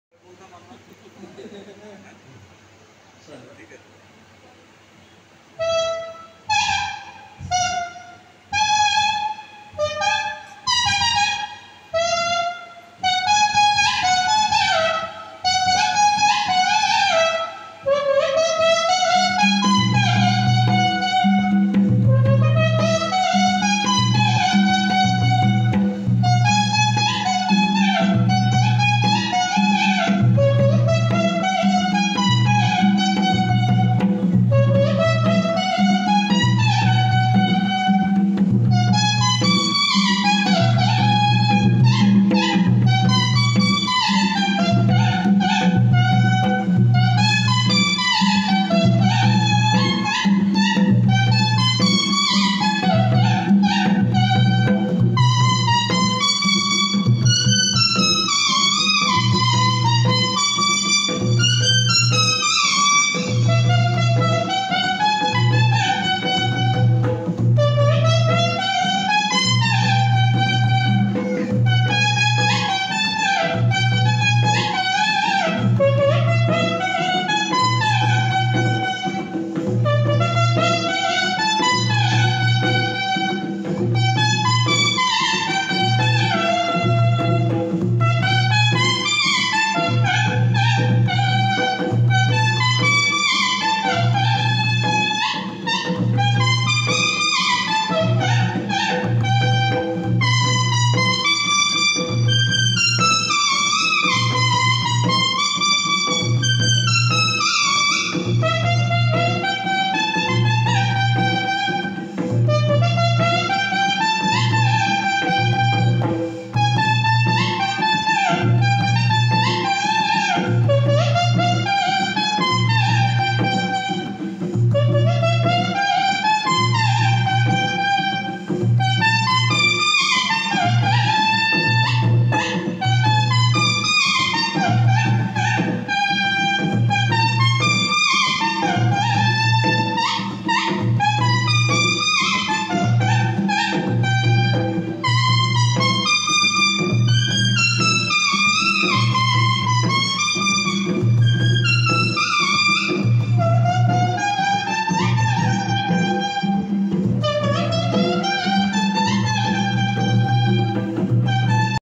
Folk Music||Tune with ''banyan leaf" by Naba Kumar Konra|| Rare talent// Hool Diwas 2022 celebration
Folk music tune cover by multi-talented Naba Kumar Konra.